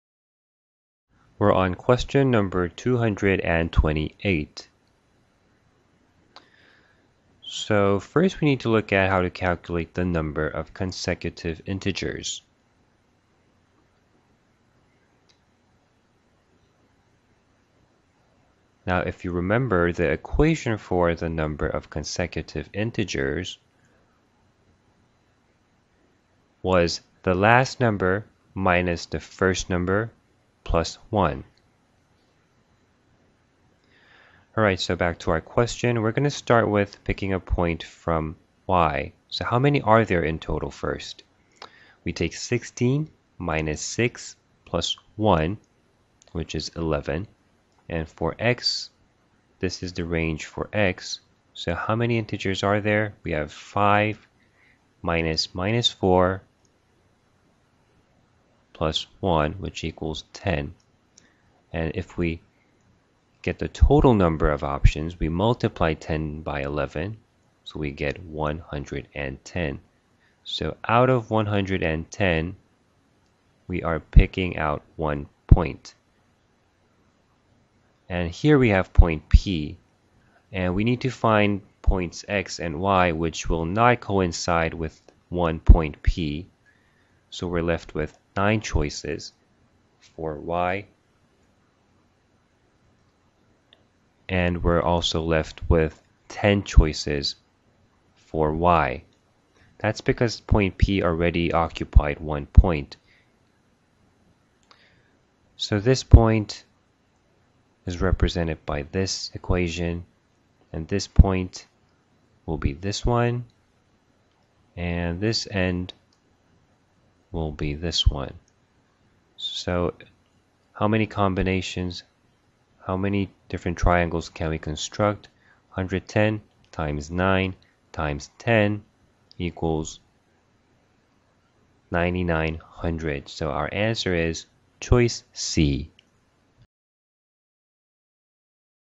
GMAT Official Guide Question PS 228 OG 2016 Video Solution
Video Explanation of the Question 228 (Problem Solving) from the GMAT Official Guide 2016
Previously this question was included in the GMAT Official Guide 2015 as Question 228 (Problem Solving)